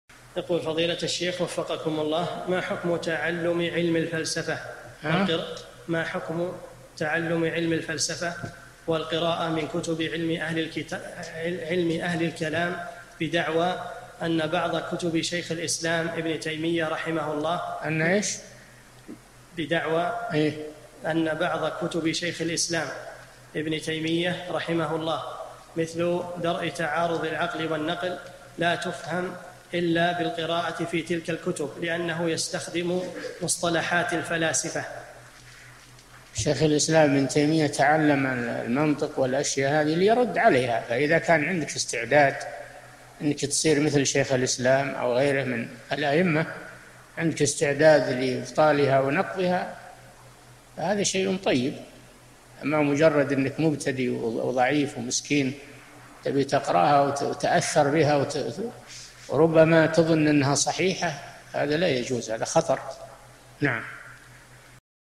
[213 /1350] ما حكم تعلم علم الفلسفة لأجل فهم بعض كتب شيخ الإسلام لأنه يستخدم بعض مصطلحاتهم ليرد..؟
[213 /1350] السؤال: ما حكم تعلم علم الفلسفة، والقراءة من كتب علم أهل الكلام، بدعوى أن بعض كتب شيخ الإسلام ابن تيمية رحمه الله، مثل [درء تعارض العقل والنقل] لا تفهم إلا بالقراءة في تلك الكتب؛ لأنه يستخدم مصطلحات الفلاسفة؟
 الجواب: شيخ الإسلام ابن تيمية تعلَّم المنطق والأشياء هذه ليرد عليها، فإذا كان عندك استعداد إنك تصير مثل شيخ الإسلام أو غيره من الأئمة، عندك استعداد لإبطالها ونقدها؛ فهذا شيءٌ طيب، أما مجرد إنك مبتدئ وضعيف ومسكين، تبي تقرأها وتتأثر بها، ربما تظن إنها صحيحة؛ هذا لا يجوز، هذا خطر.
للاشتراك في قناة الشيخ صالح الفوزان
أسئلة وأجوبة من شرح قاعدة جليلة ( 1350 ) مقطع

للاستفسار والإقتراحات والدعم
رئيس الفريق
لمشروع كبار العلماء